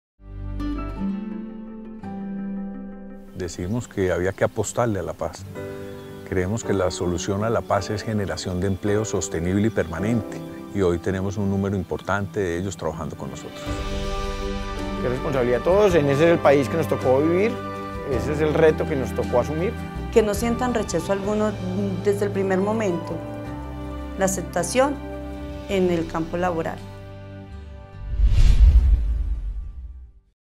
Una oportunidad de Casa Grajales y Tecnicafe que lo cambia todo.
Ellos cambiaron paradigmas para la inclusión laboral de las personas que dejaron las armas, eso es